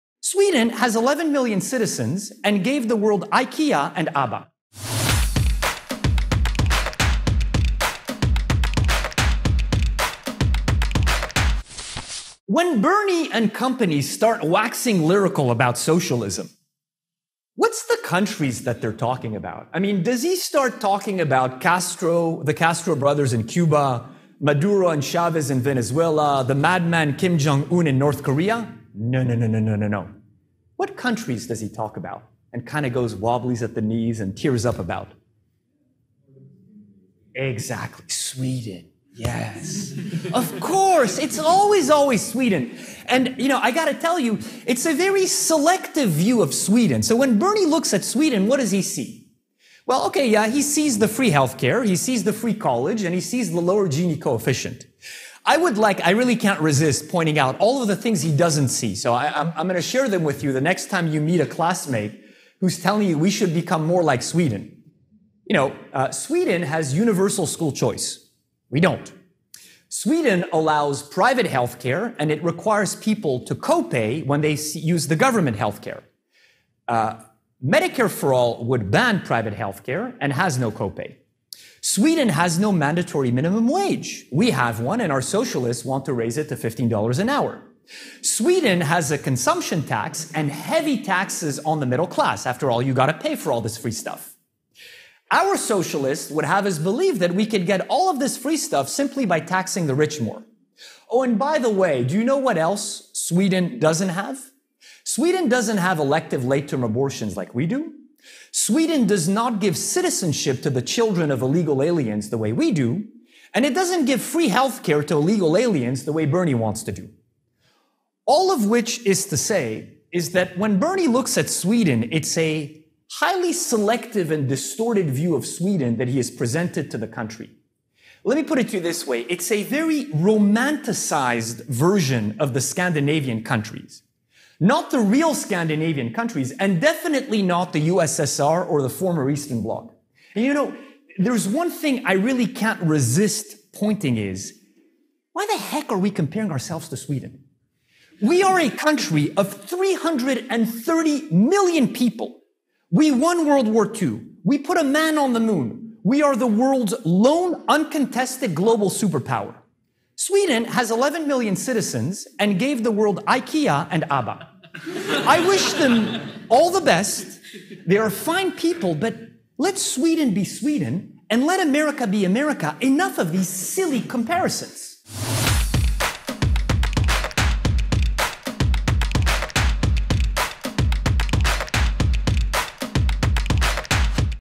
NOT SO FAST: David Azerrad rebukes Bernie's odd crush on Sweden
America has 330 million citizens, won WWII, put a man on the moon, and is the world's lone, uncontested superpower. Why are we trying to be like Sweden?